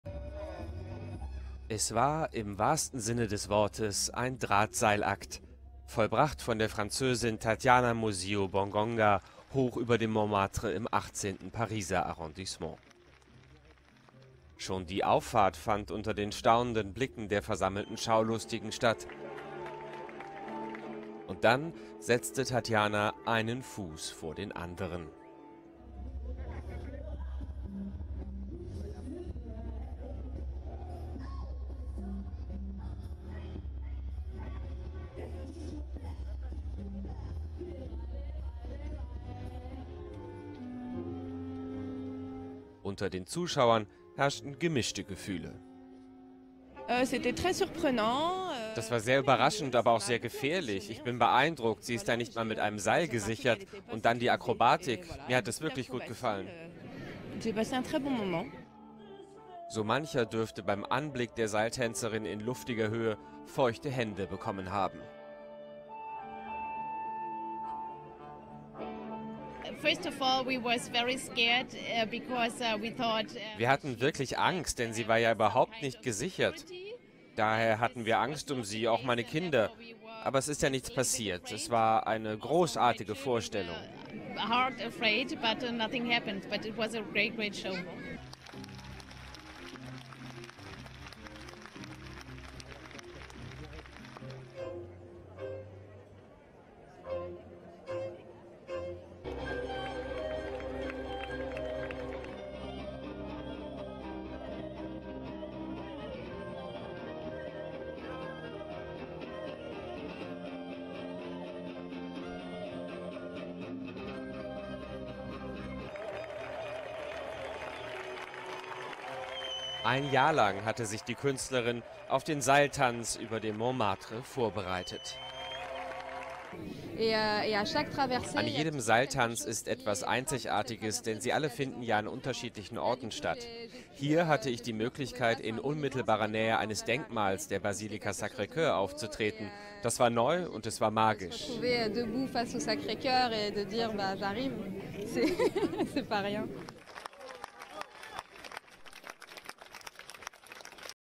Drahtseilakt über den Dächern von Paris
Nach einem Jahr Vorbereitung ließ die Seiltänzerin vielen Passanten den Atem stocken.